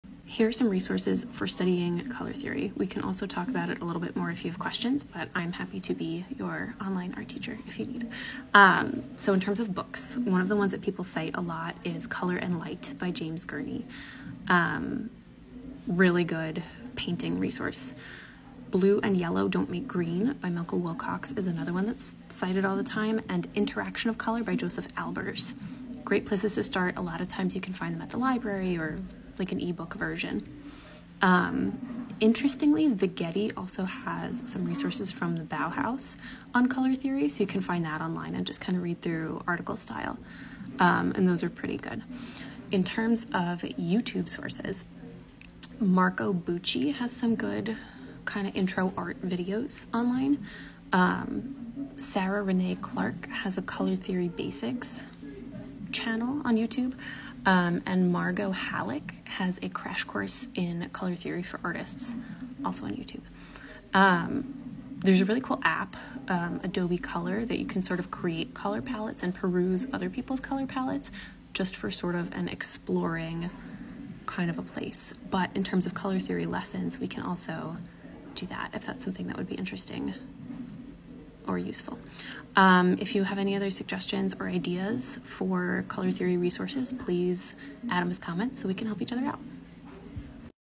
Color Theory Resources for Beginners | Art Teacher and Illustrator Daily Vlog
Replying to @ Becky Dee

Resources for studying color theory! Let me know if you have any questions, or suggestions for more resources!!

James Gurney "Color and Light"

Michael Wilcox "Blue and Yellow Don't Make Green"

Josef Albers "Interaction of Color"

Marco Bucci color - youtube

Sarah Renae Clark - color theory basics - youtube

Margot Hallac - a crash course in color theory for artists - youtube

Adobe Color